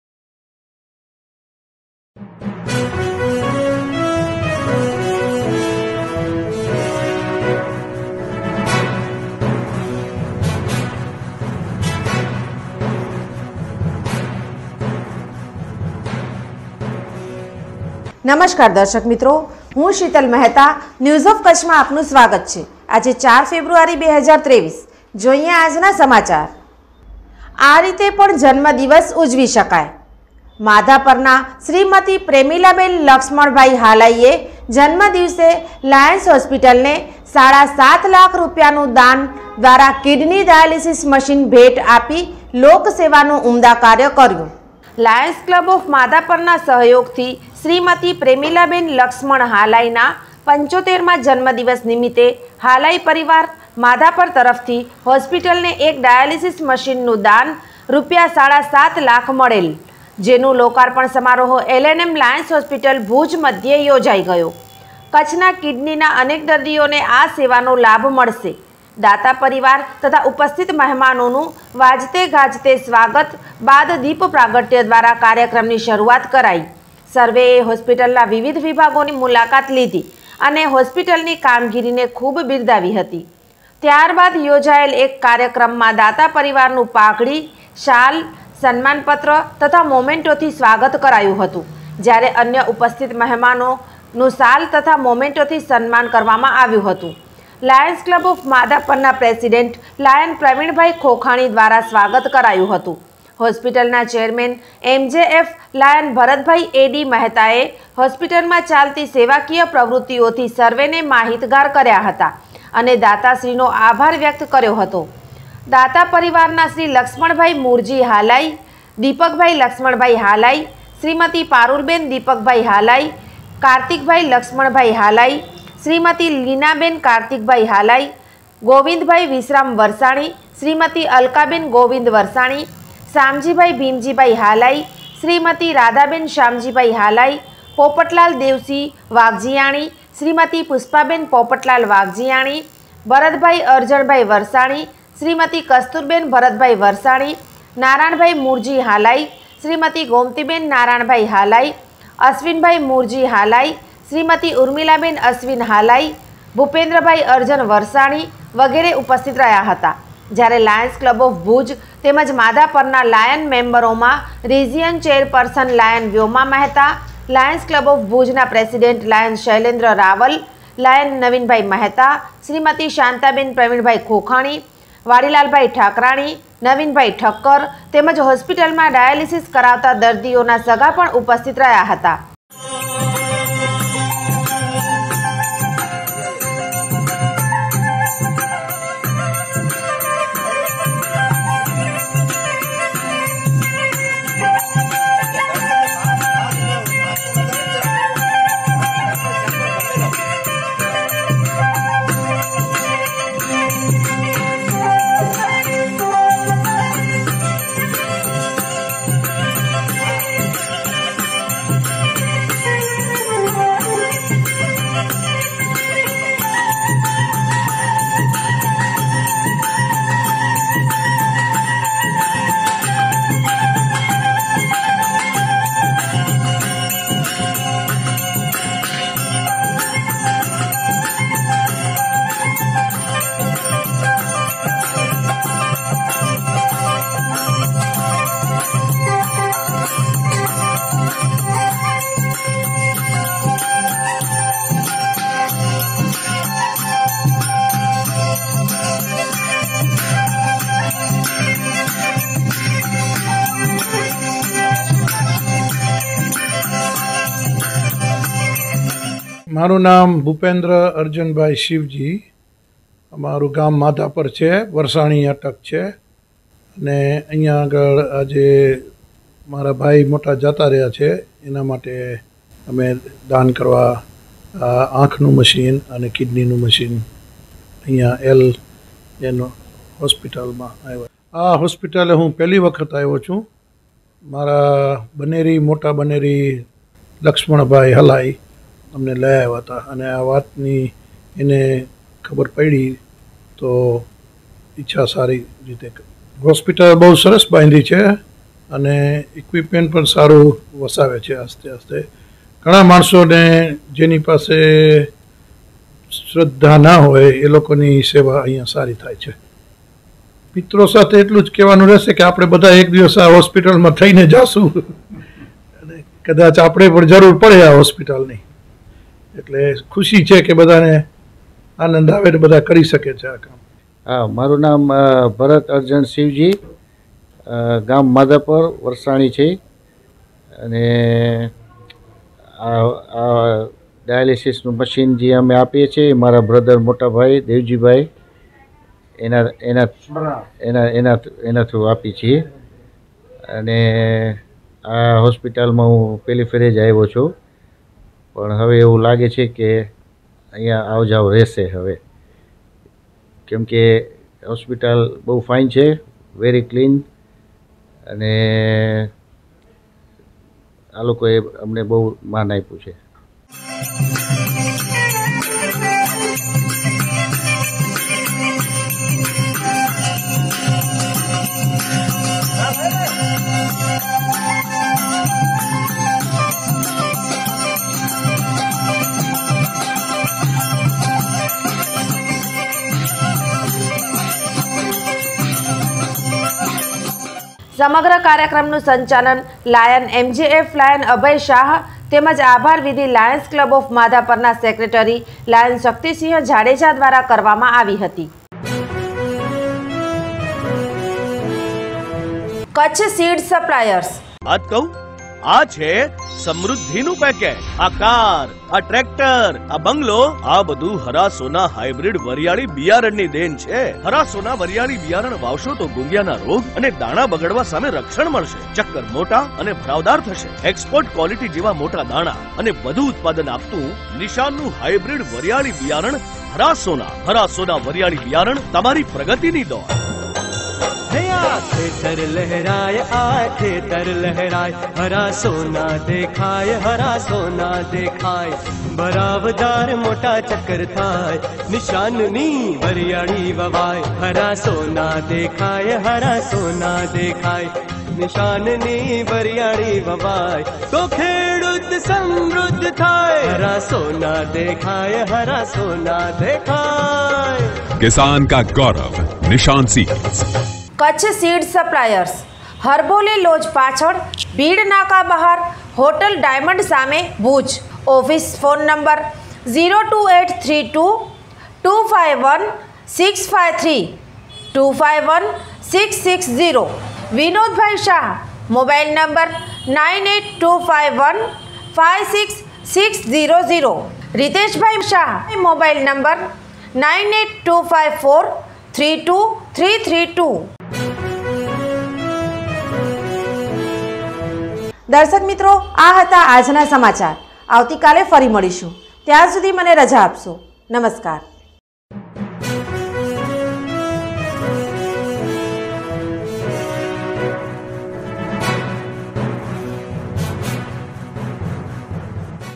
માધાપરના પ્રેમીલાબેને લાયન્સ હોસ્પિટલને સાડા સાતલાખની કિંમતનું દાન આપી લોકસેવાનું ઉમદા કાર્ય કર્યું🌹
આ રીતે પણ જન્મદિવસ ઉજીવી શકાય છે માધાપરના પ્રેમીલાબેને લાયન્સ હોસ્પિટલને સાડા સાત લાખની કિંમતનું દાન આપી લોકસેવાનું ઉમદા કાર્ય કર્યું🌹